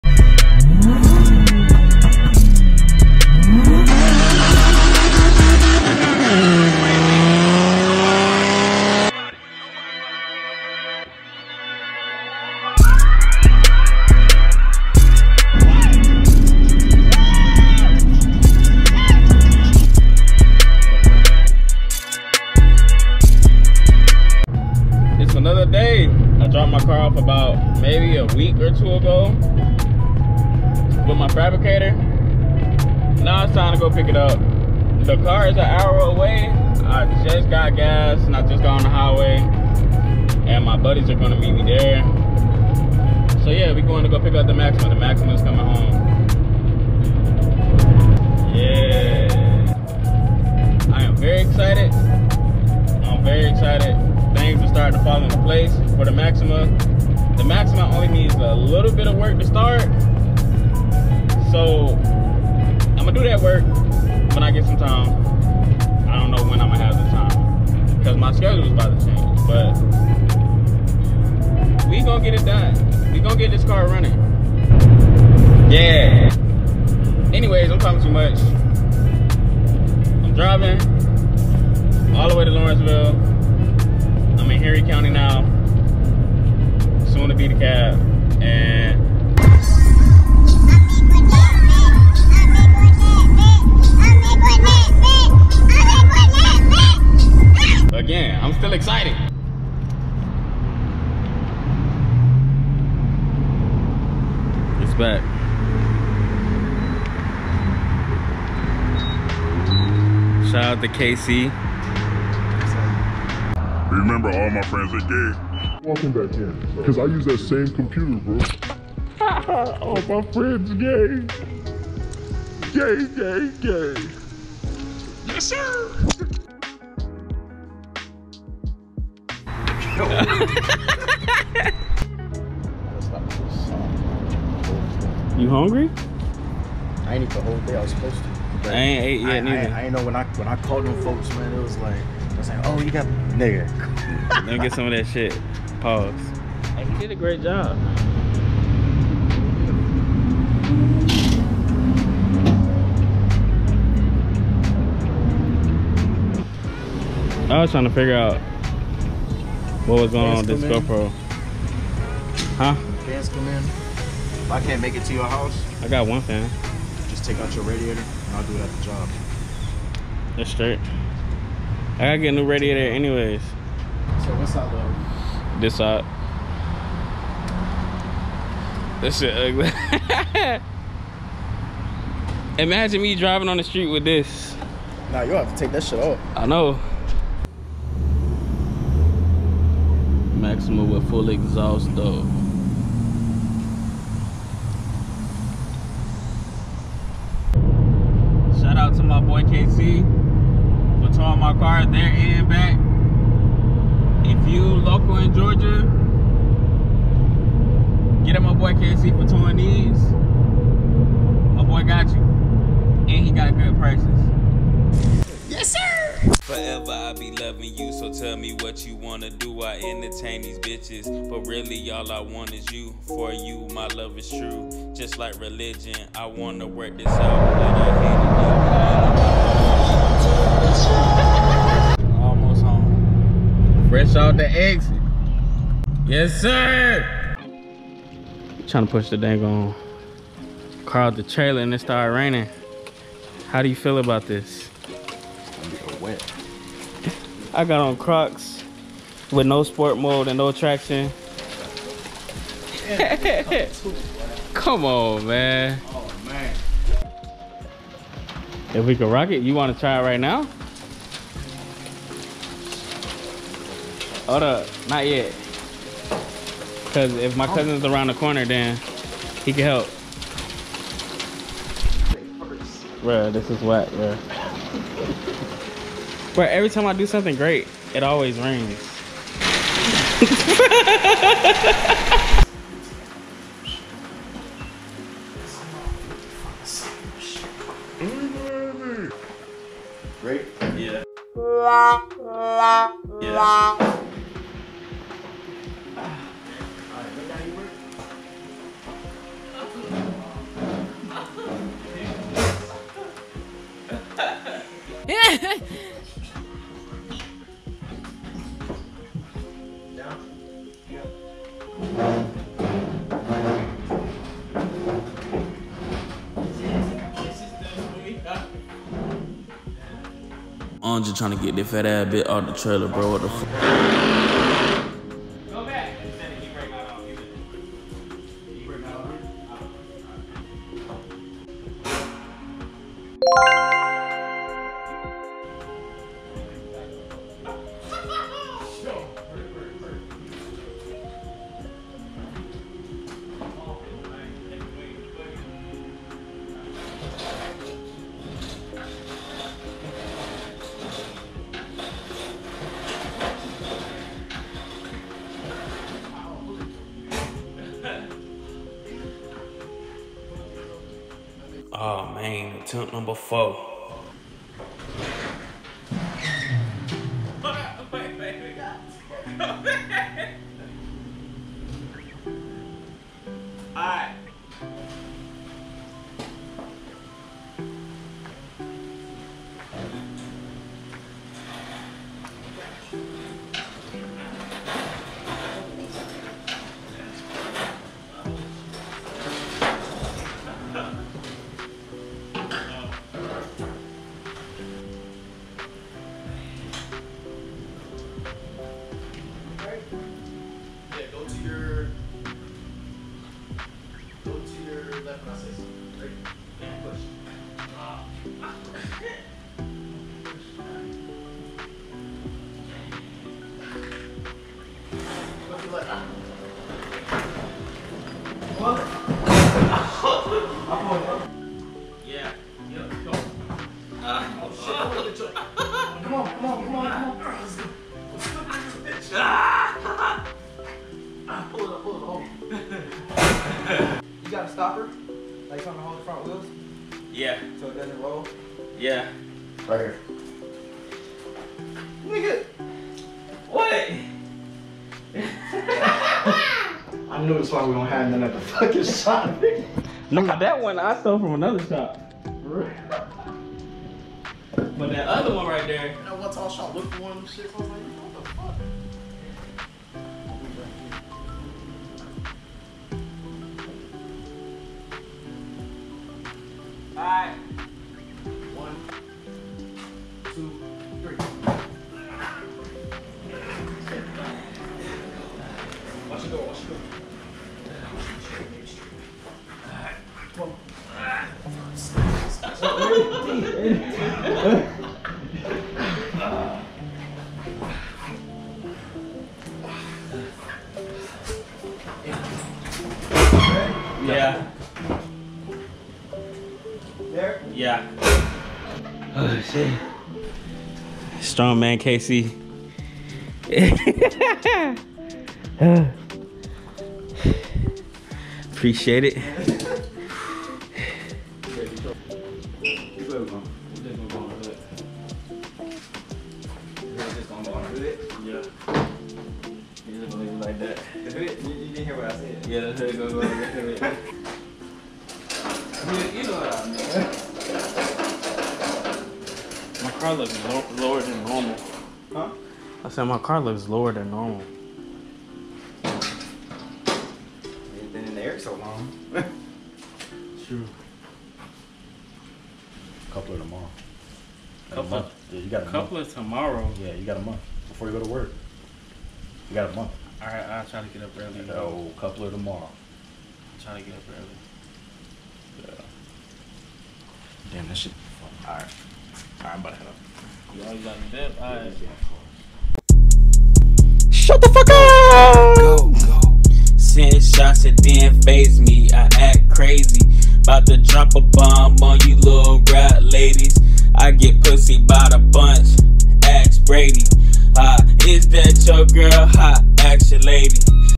Turbo Maxima Towed Back From The Fabricator
As promised part 2 is here !
In this video my boy KC and I are going to pick up my maxima from the fabricator .
The fabricator installed the intercooler and the intercooler piping and also made me a custom full exhaust so that I can be a little sleeper . It’s a breakaway full exhaust so if I want to take it off and run a hood exit I can or if I want to take it off and have it dump under the transmission, I can ! I’m very excited to have my car back . Was definitely happy with the quality and the help from KC and Xayy ! Shoutout to my fabricator JAH FAB . I will be leaving his information lower down in the description!

Contact Xay

Contact KC

Contact JAH FAB